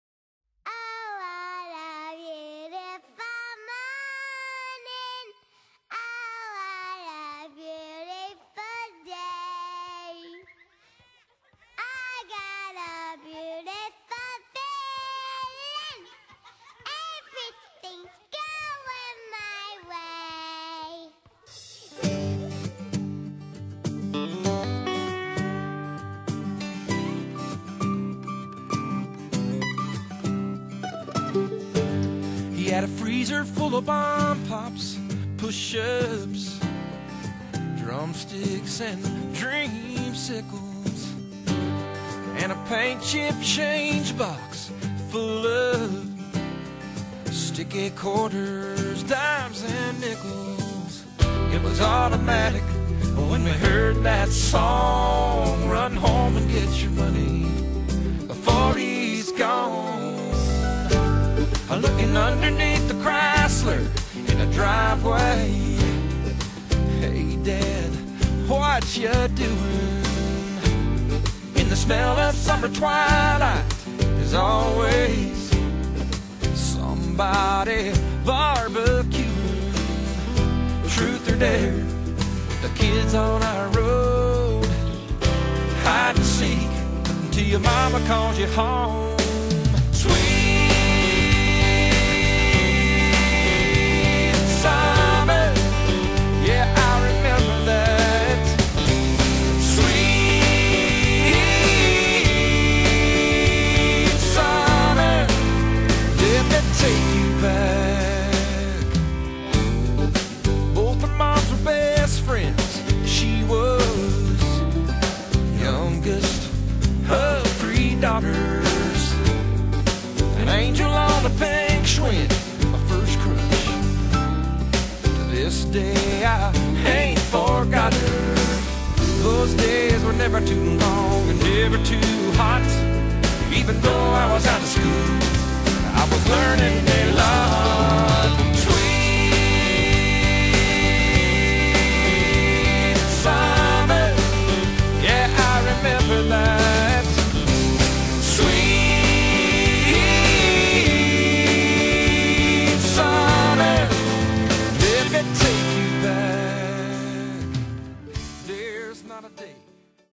Sweet Summer
Ana in July 2008  enjoying the days of summer